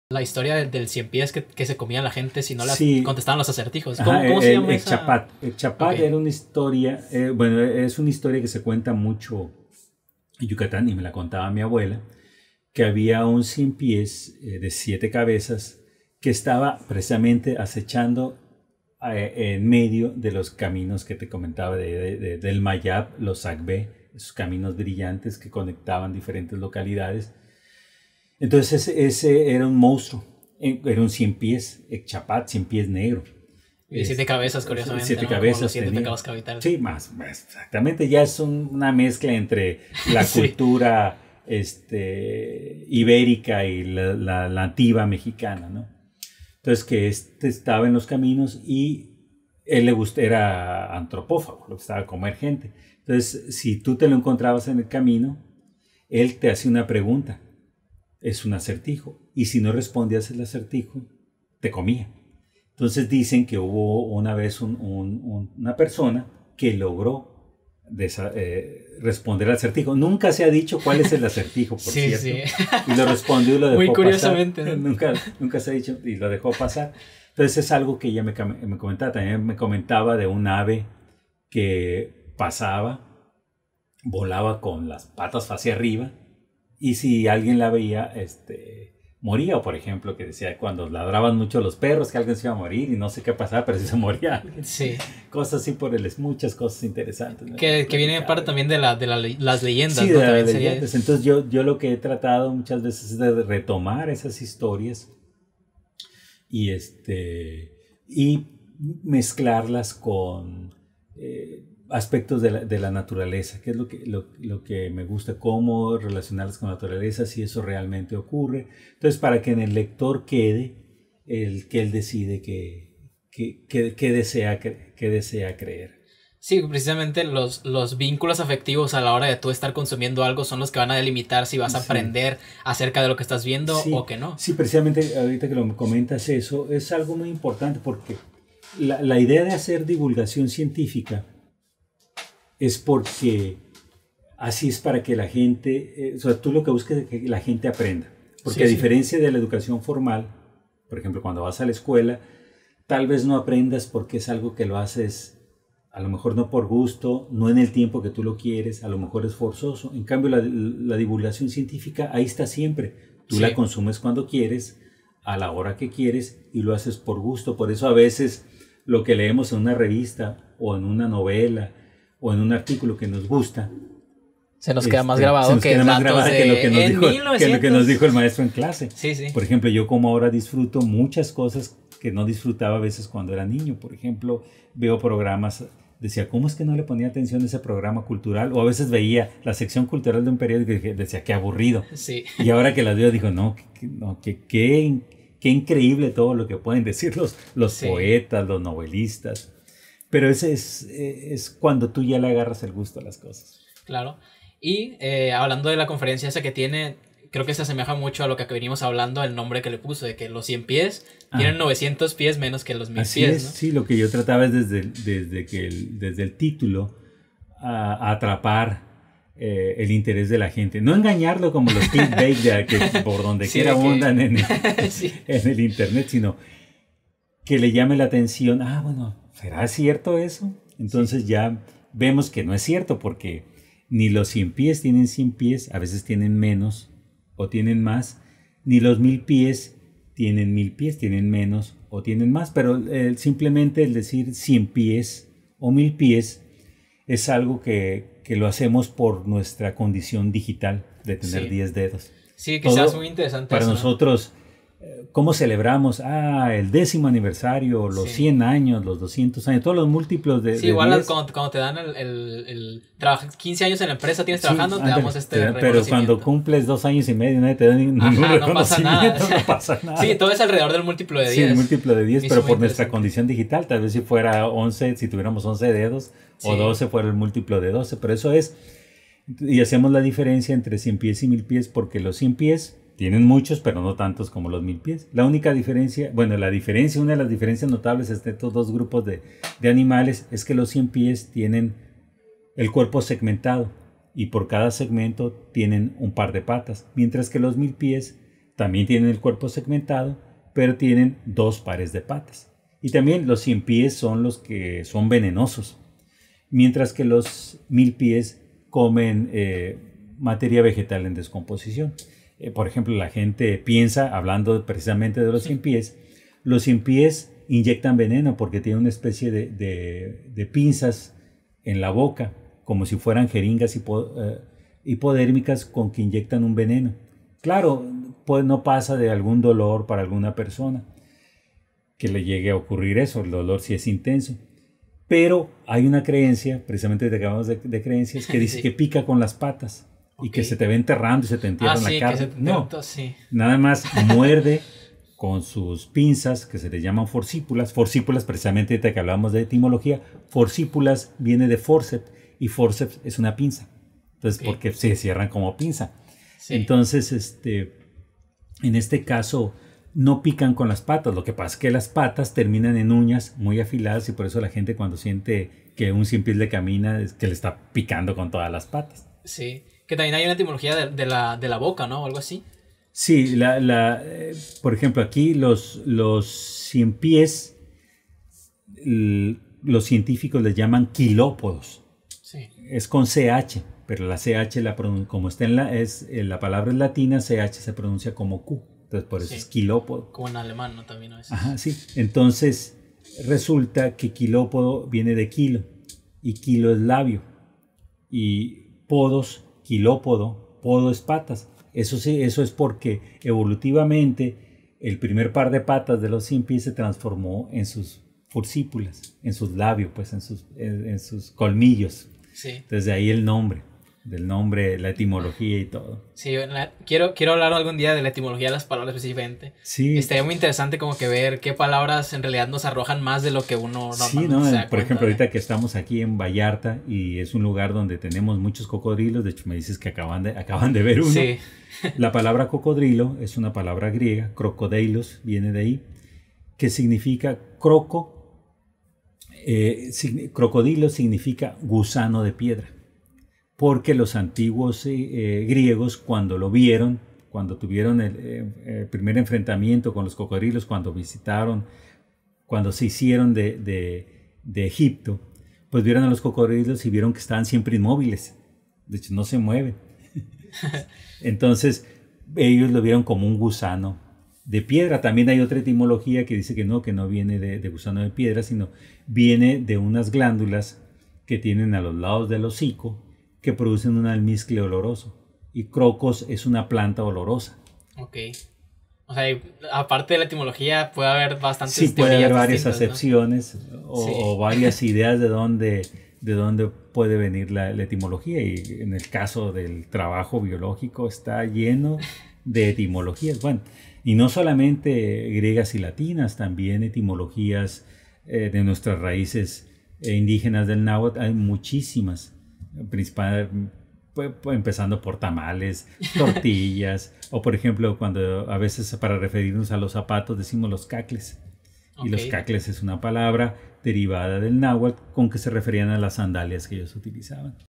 LEYENDAS LEGENDARIAS CON FABIO CUPUL II ESTE PODCAST YA EXISTE (O NO) II
El Ek Chapat y la narrativa de las historias antiguas para crear ficcón en la divulgación científica.